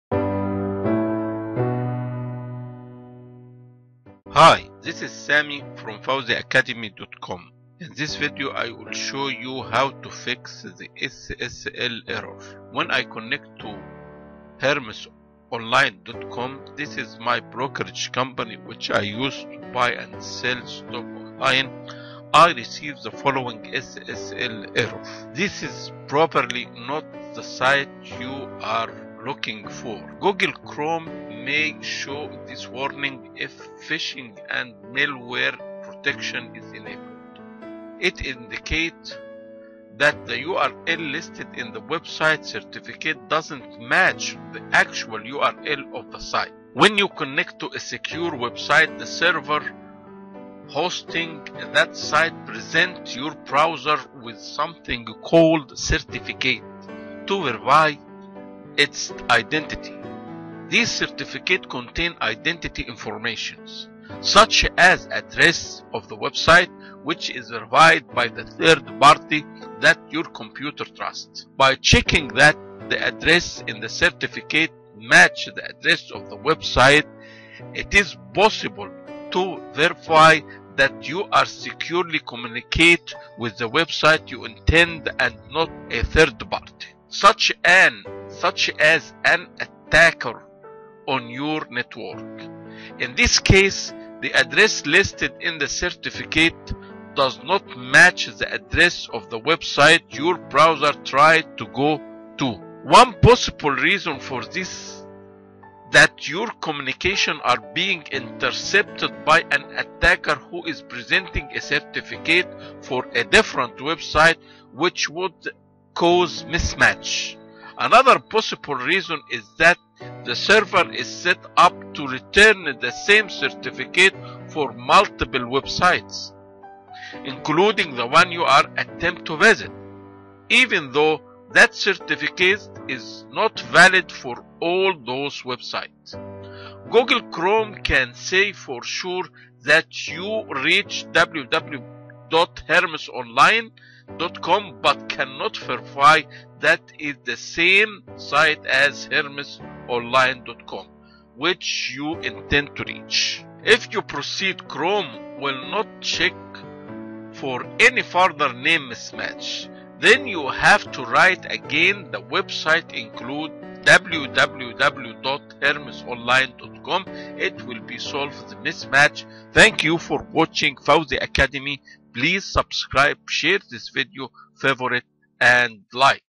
SSL connection error, How to fix http error, SSL protocol error
this video talks about SSL connection error , what is SSL error,SSL certificate error, security certificate error

Fawziacademy use google translator as and google translate app to translate & voice